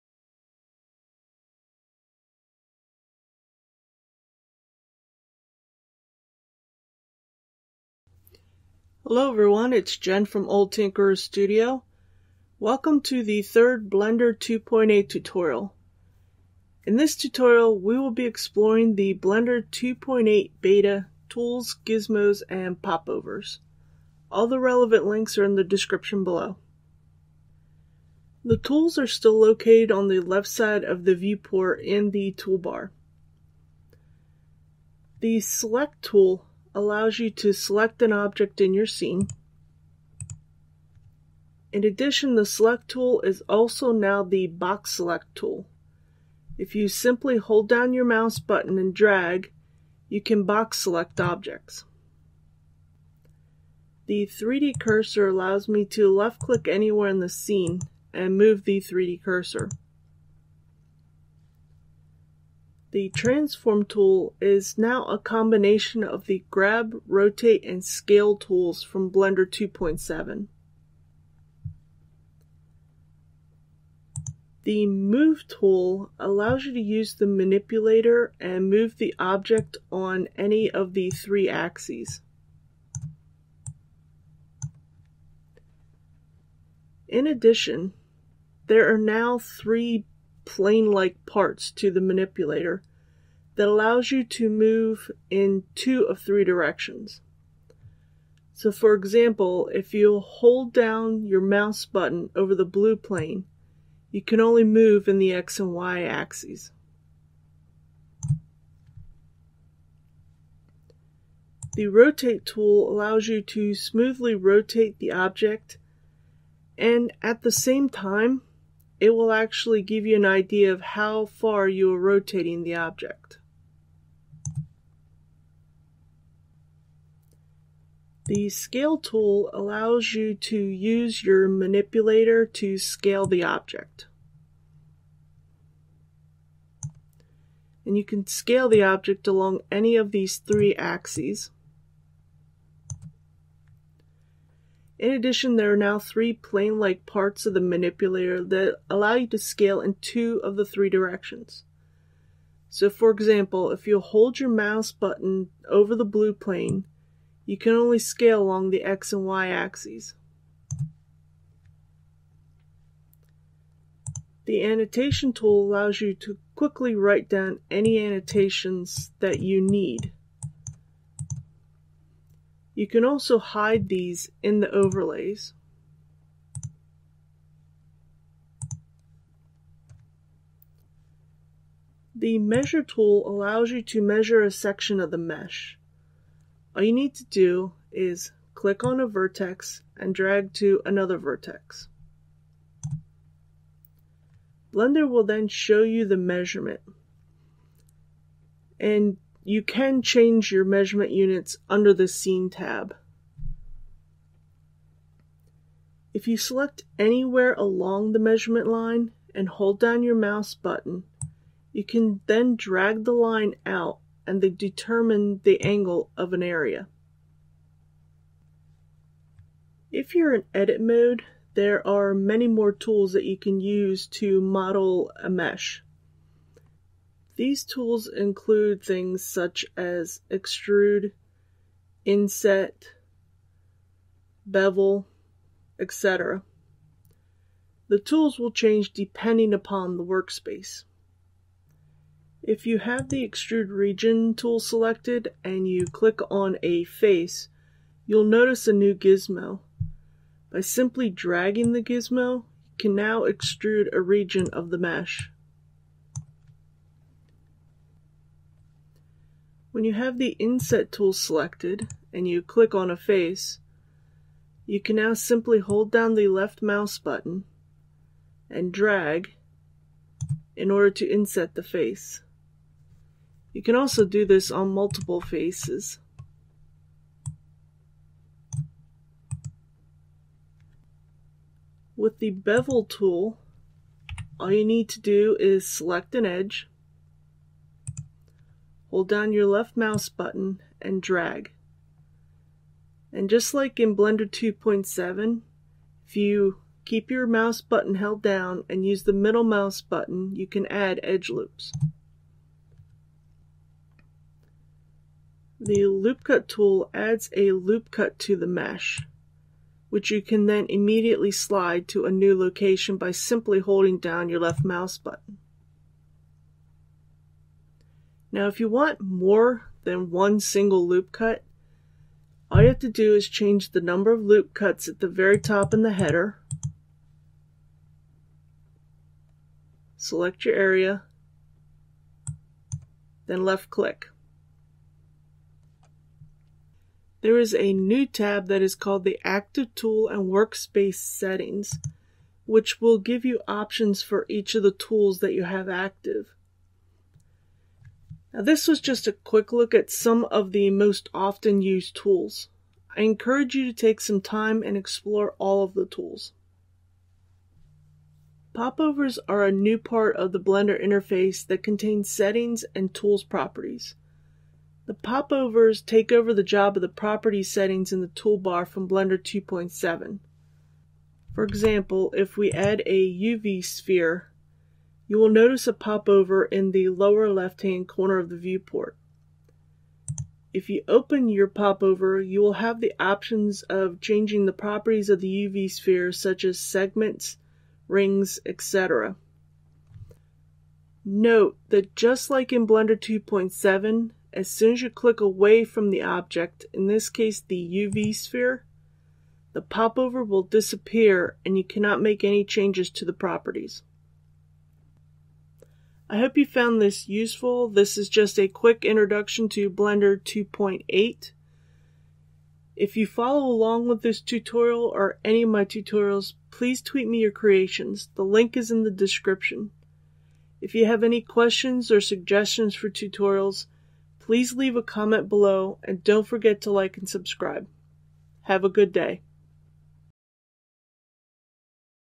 [Blender 2.8] How to use the tools, gizmos, and popovers in Blender | Blender 2.8 Tutorial
Welcome to the third Blender 2.8 tutorial. In this tutorial we will be exploring the Blender 2.8 Beta tools, gizmos, and popovers.

Timestamps:
Tools 0:00:27
Gizmos 0:03:39
Popovers 0:05:56

I am a CG Artist focusing on environments, props, and textures. In addition, I also work with visualizations, cinematics, and filmmaking.